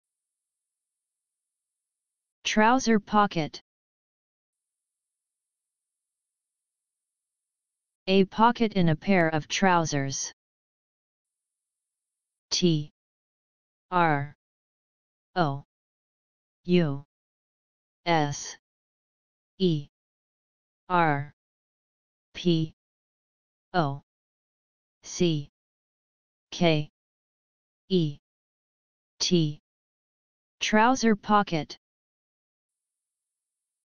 what is the meaning of trouser pocket.
what trouser pocket means:
A pocket in a pair of trousers.